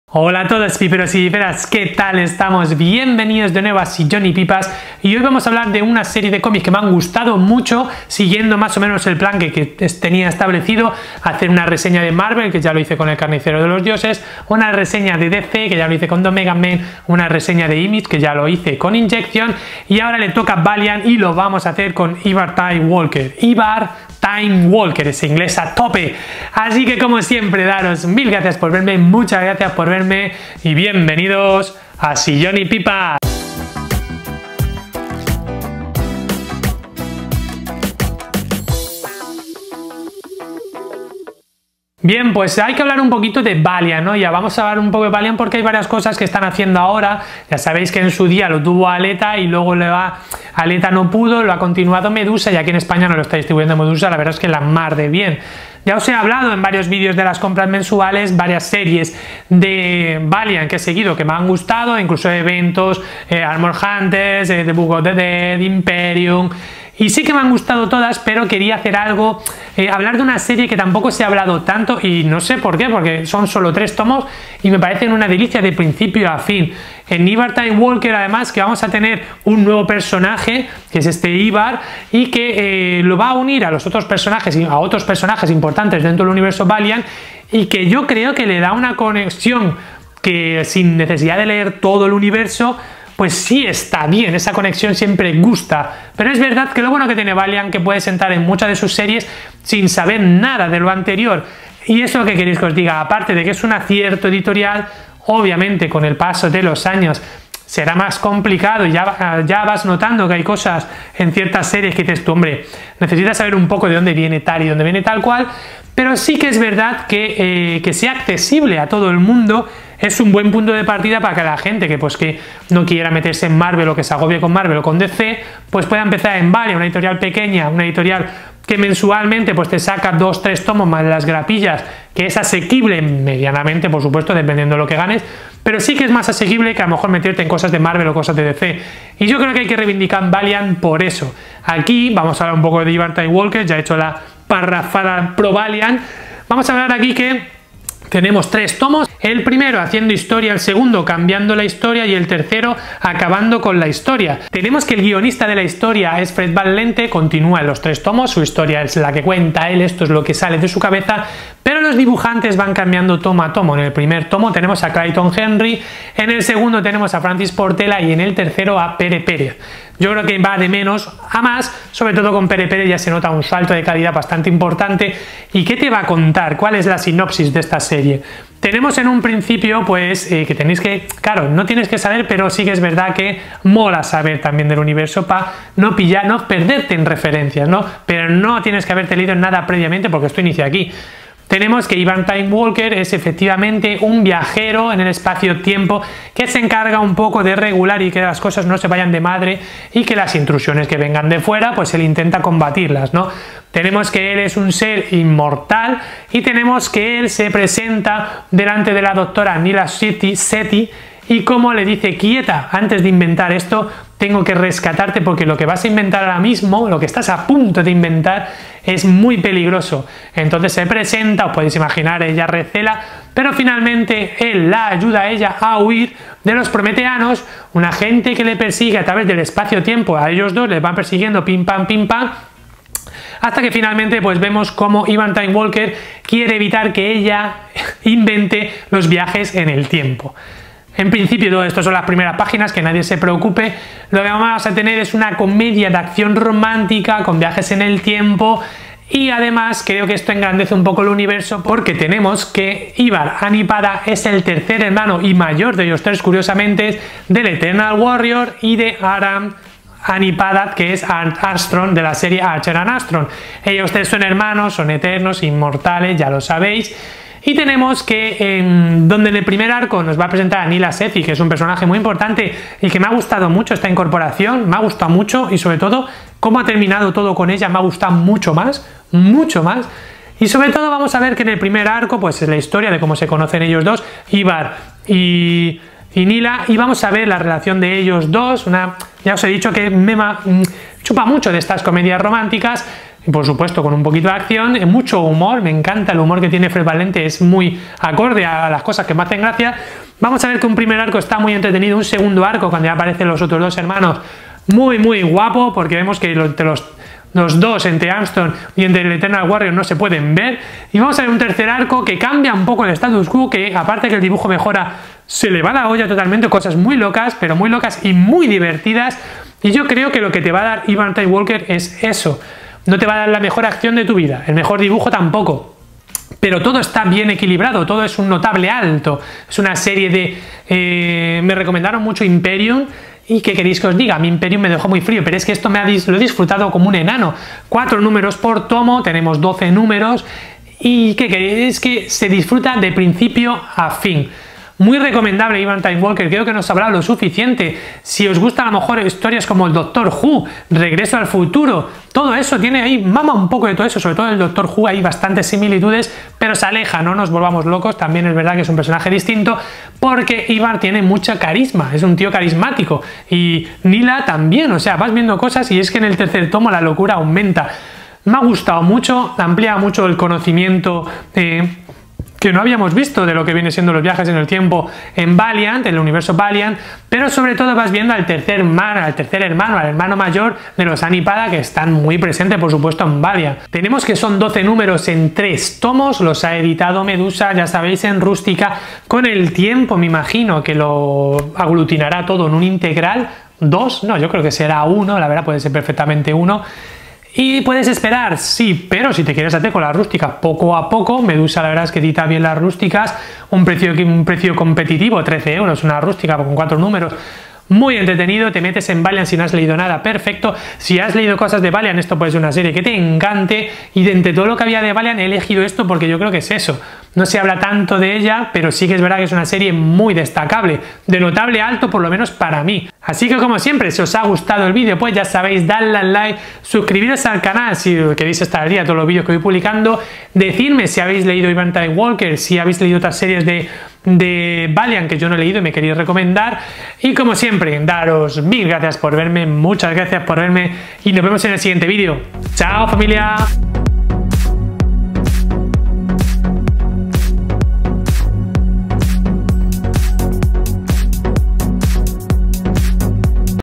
Reseña Ivar Timewalker / Valiant Comics / Medusa Editorial / Comic Book
Video reseña de los tres tomos de Ivar Timewalker, una serie del Universo Valiant, en la que descubriremos los viajes en el tiempo y al mayor de los tres hermanos Anni-Padda, ¡entretenimiento en estado puro!